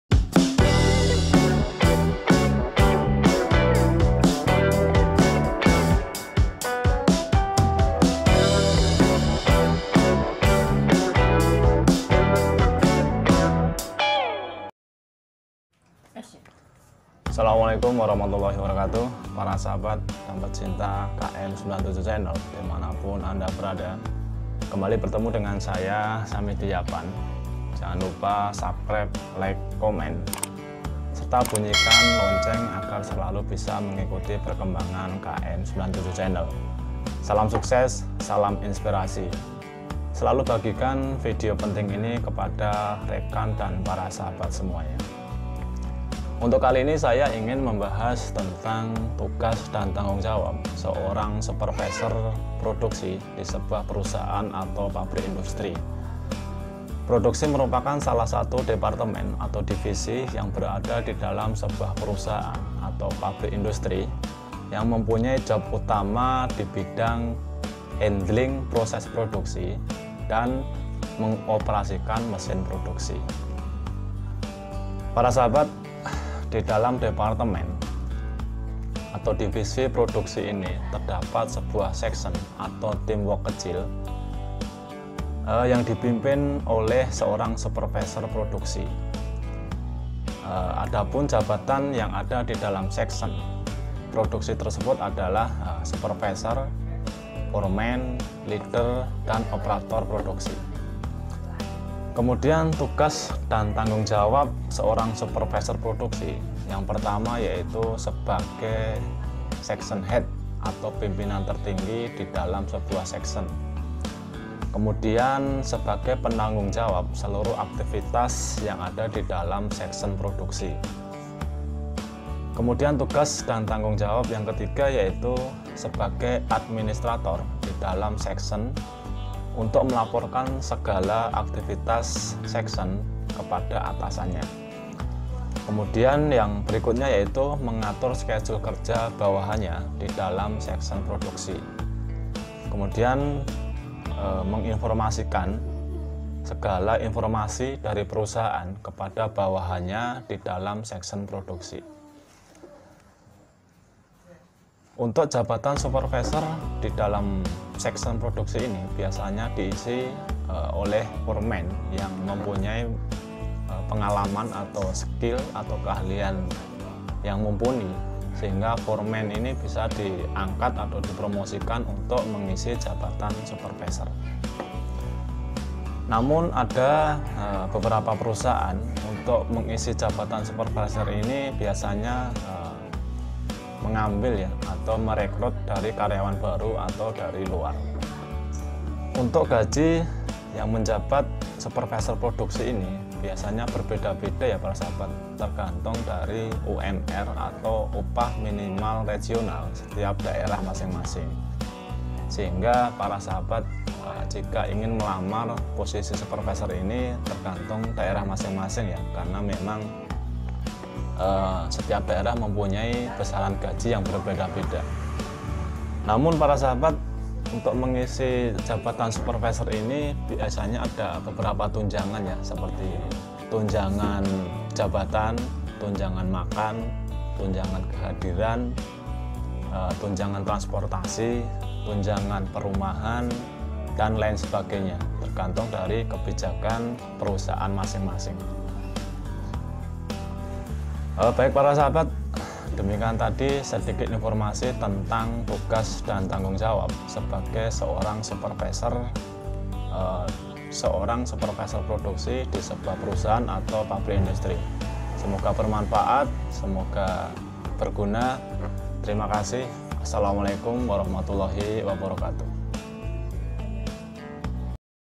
Tugas Supervisor Produksi di Perusahaan Versi KM 97 Channel
Assalamualaikum wr.wb

Para sahabat Km97Channel di manapun anda berada..pada video kali ini kami membahas tentang Tugas Jabatan Supervisor produksi di perusahaan pabrik Industri.

Tugas dan tanggung jawab Supervisor produksi di perusahaan pabrik Industri adalah sebagai Section head atau penanggung jawab team work di section produksi.

Jabatan supervisor Produksi perusahaan adalah membawahi jabatan foreman produksi, jabatan leader produksi, jabatan operator produksi di pabrik Industri.

Seorang Jabatan supervisor Produksi bertugas mengontrol semua pekerjaan di lapangan.

Lebih lengkapnya tonton sampai habis video  km 97 channel..